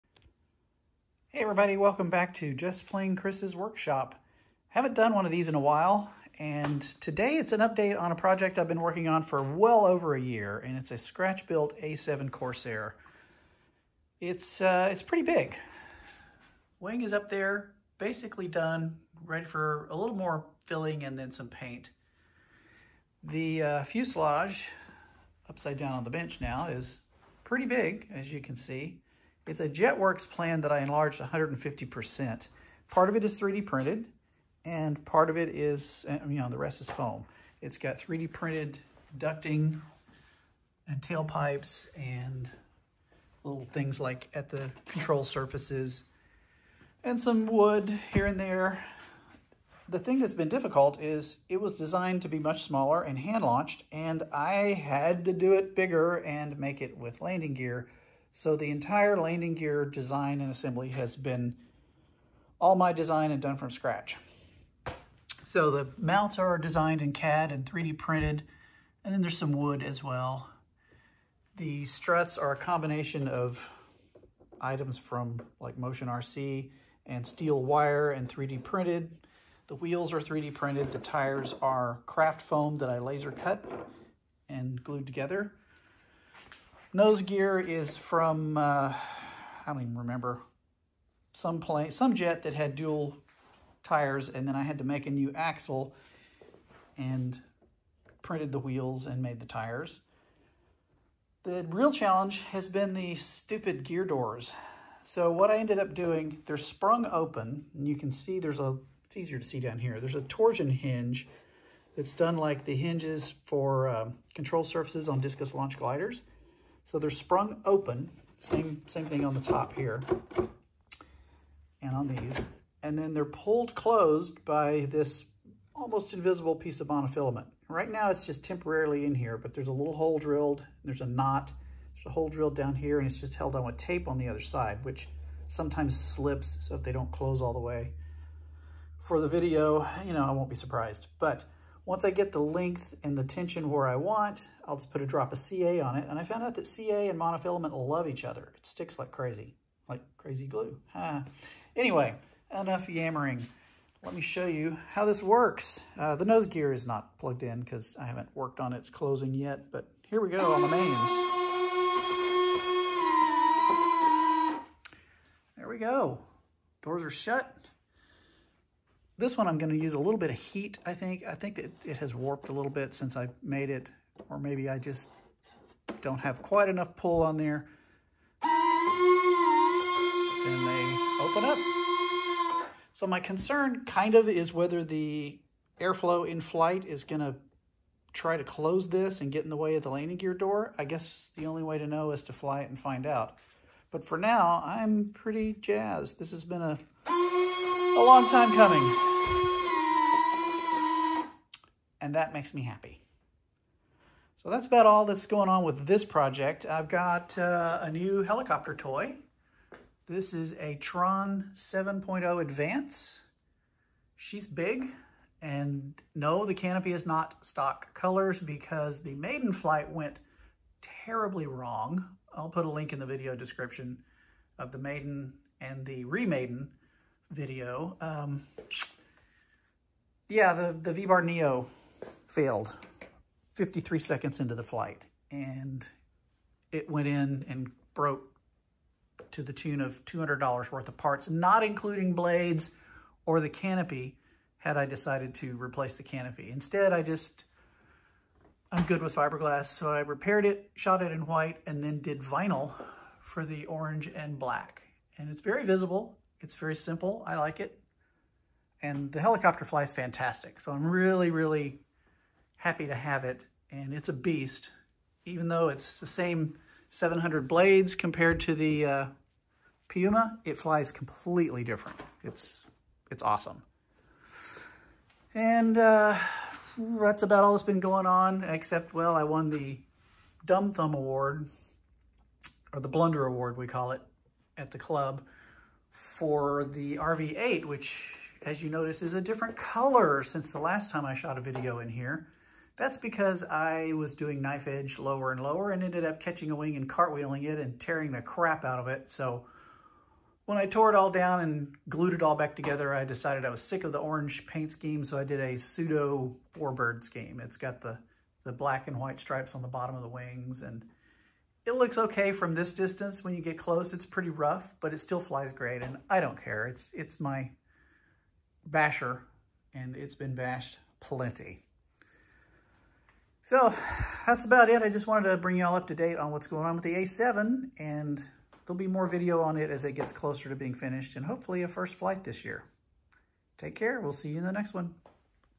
JPC’s Workshop: February 9, 2025
Landing gear design on the scratch built Jetworks A-7 Corsair II, and some heli news.